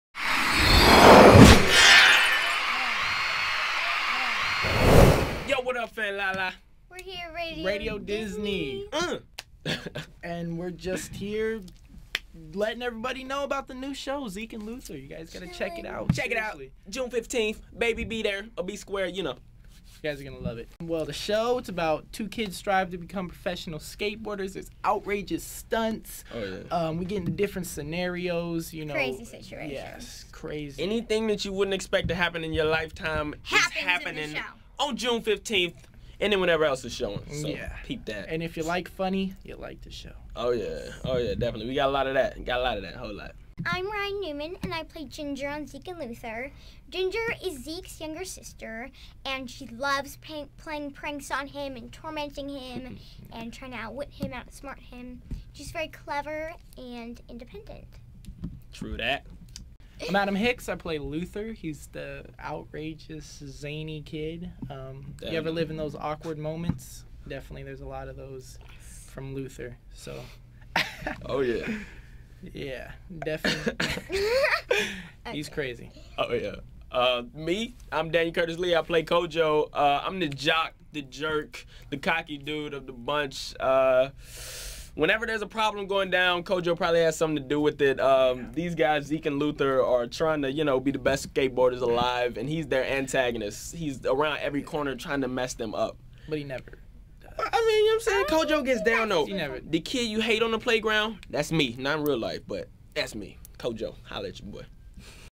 Zeke and Luther cast at Radio Disney
Adam Hicks, Daniel Lee Curtis, and Ryan Newman take over Radio Disney and give you a sneak peak at their new show "Zeke and Luther" premiering on Disney XD on June 15.